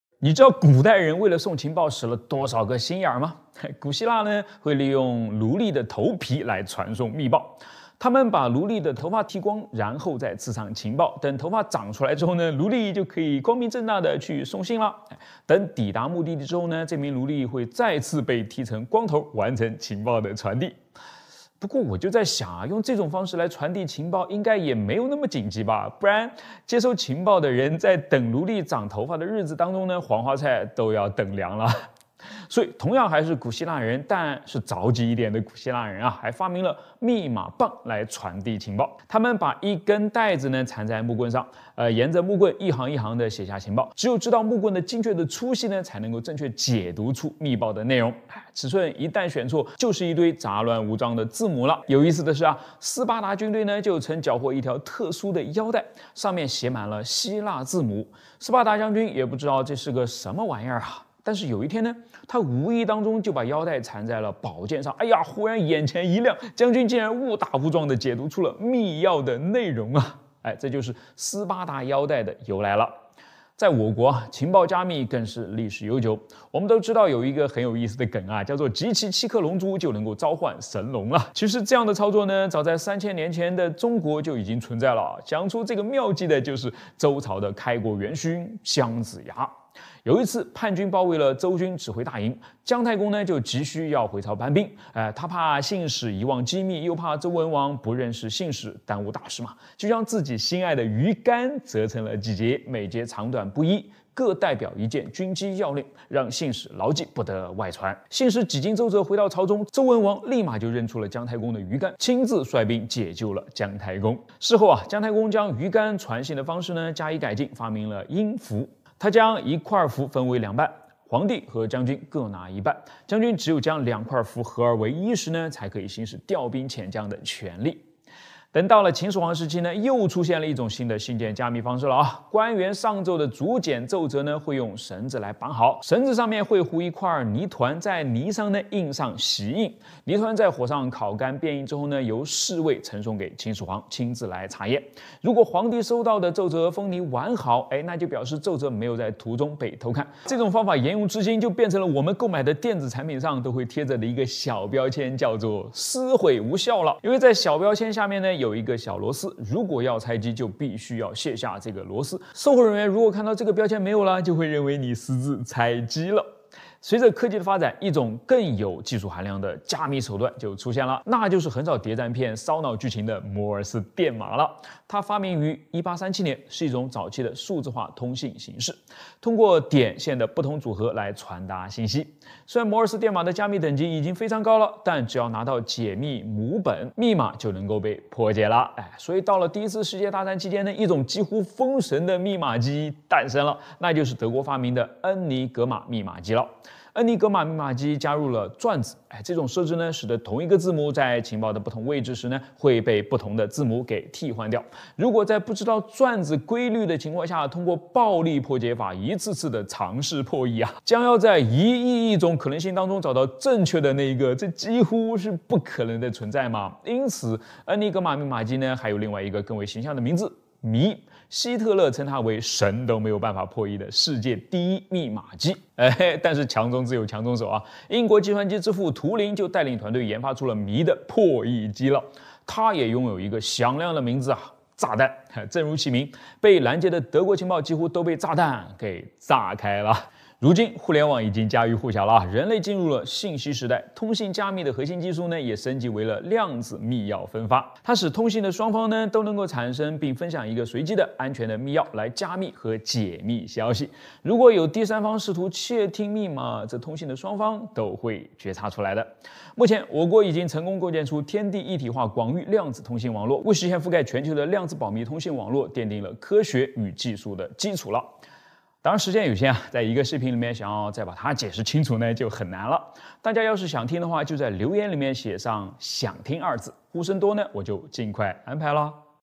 用头皮送信，古代人为了送情报能有多拼！【科学火箭叔】
本频道制作的所有视频都是为了向观众传递有趣的科普知识，绝对符合平台的使用政策。 大家好，我是你们的“百科全叔”火箭叔~